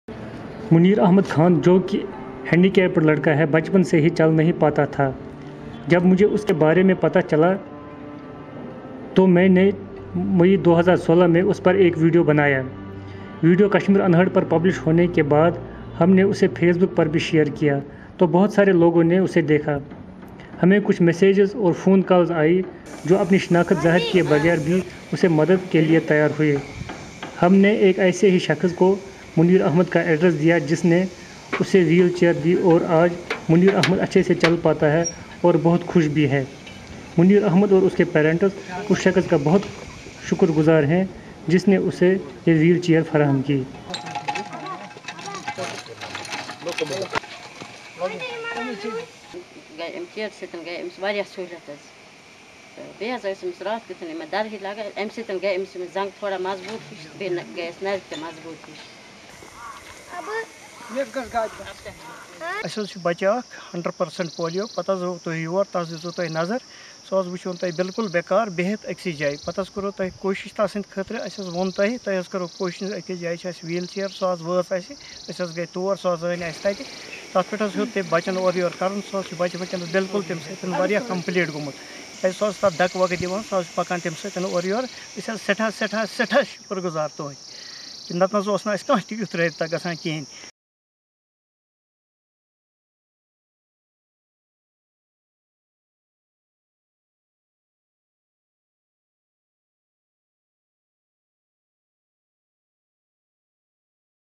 IMPACT: Differently Abled Boy Gets Free Wheelchair As Netizens Converge To Assist
Muneer Ahmad, a 16 years old, differently abled boy had to face lots of inconvenience in moving from one place to another. He cannot walk properly and was in need of a wheel chair. Belonging to a poor family his parents could not afford the same for him. But,
after the video story about the plight of this boy was published by Video Volunteers on its website, Kashmir Unheard, it was shared on several social networking sites and many people came forward to help this boy. A wheelchair was then provided to him recently by one of the users who had pledged to assist this boy. This not only brought convenience for the movement of Muneer but joys also. Mir Yaseen Reports